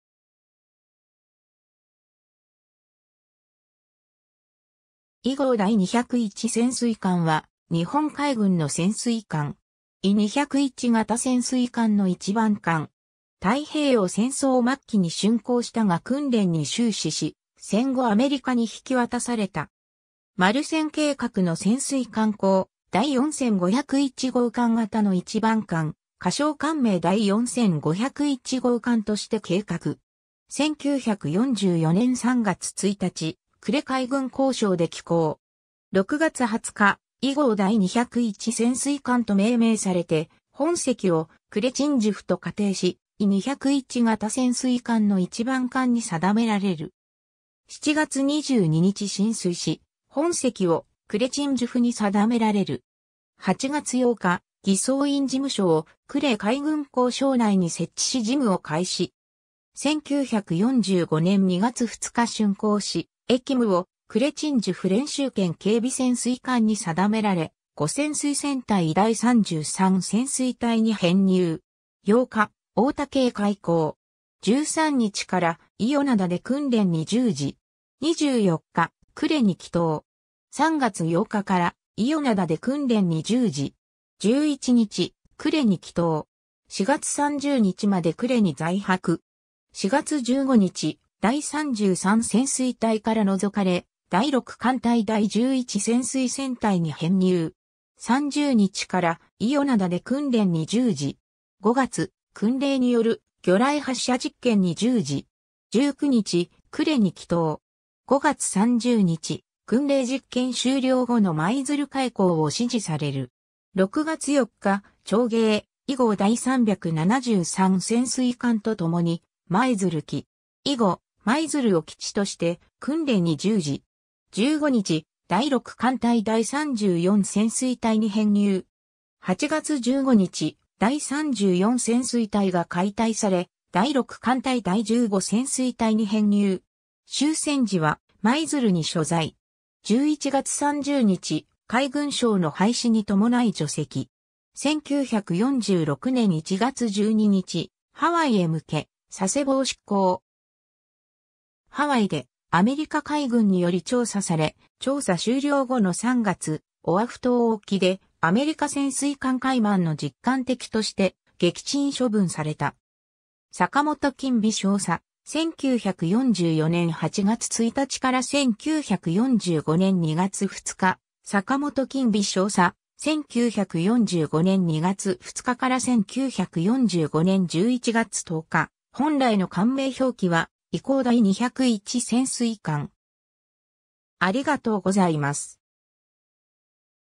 伊号第二百一潜水艦
伊号第二百一潜水艦（いごうだいにひゃくいちせんすいかん）は、日本海軍の潜水艦。
伊二百一型潜水艦の1番艦。
太平洋戦争末期に竣工したが訓練に終始し、戦後アメリカに引き渡された。
 マル戦計画の潜水艦高、第4501号艦型の1番艦、仮称艦名第4501号艦として計画。
 1944年3月1日、呉海軍工廠で起工。
6月20日、伊号第二百一潜水艦と命名されて本籍を呉鎮守府と仮定し、伊二百一型潜水艦の1番艦に定められる。
7月22日進水し、本籍を呉鎮守府に定められる。
8月8日、艤装員事務所を呉海軍工廠内に設置し事務を開始。
 1945年2月2日竣工し、役務を呉鎮守府練習兼警備潜水艦に定められ、呉潜水戦隊第三十三潜水隊に編入。
8日、大竹へ回航。
13日から伊予灘で訓練に従事。
24日、呉に帰投。
 3月8日から伊予灘で訓練に従事。
11日、呉に帰投。
4月30日まで呉に在泊。
 4月15日、第三十三潜水隊から除かれ、第六艦隊第十一潜水戦隊に編入。
30日から伊予灘で訓練に従事。
 5月、訓令による魚雷発射実験に従事。
19日、呉に帰投。
5月30日、訓令実験終了後の舞鶴回航を指示される。
 6月4日、長鯨、伊号第373潜水艦とともに舞鶴着。
以後、舞鶴を基地として訓練に従事。
15日、第六艦隊第三十四潜水隊に編入。
 8月15日、第三十四潜水隊が解隊され、第六艦隊第十五潜水隊に編入。
終戦時は舞鶴に所在。
11月30日、海軍省の廃止に伴い除籍。
 1946年1月12日、ハワイへ向け佐世保を出港。
ハワイでアメリカ海軍により調査され、調査終了後の3月、オアフ島沖でアメリカ潜水艦カイマンの実艦的として撃沈処分された。
 坂本金美 少佐：1944年8月1日 - 1945年2月2日 坂本金美 少佐：1945年2月2日 - 1945年11月10日 ^ 本来の艦名表記は伊號第二百一潛水艦。